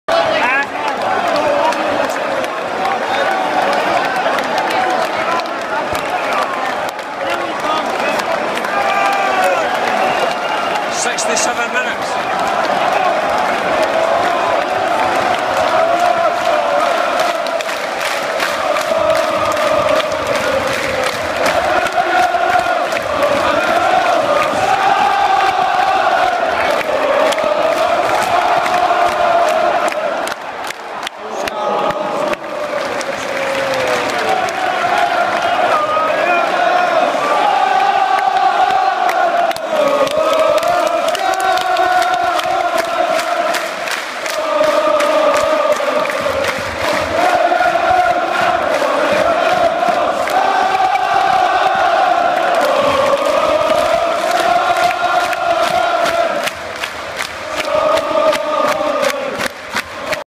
Scotland Football Fans Celebrating
Footage of supporters of the Scottish national football team celebrating after their team had equalised in a Euro 2016 qualification match between Germany and Scotland in Dortmund on 7 September 2014.